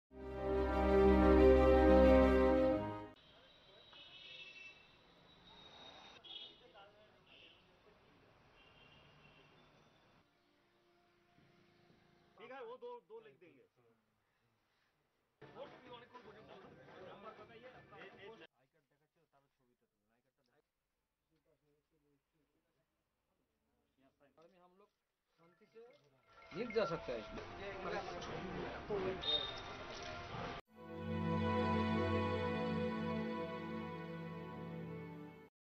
Postal Department Facilitate Soldiers To Exchange Old Currency Notes
At a time when entire country is going through monetary crisis, the Indian Postal department has come forward to help Indian Border Security Force. The postal division has set up several temporary camps along the Indo-Bangladesh Border out post (BOPs) in South Dinajpur district of West Bengal, where district postal officers are exchanging recently banned Rs. 500 and Rs. 1,000 notes of the soldiers.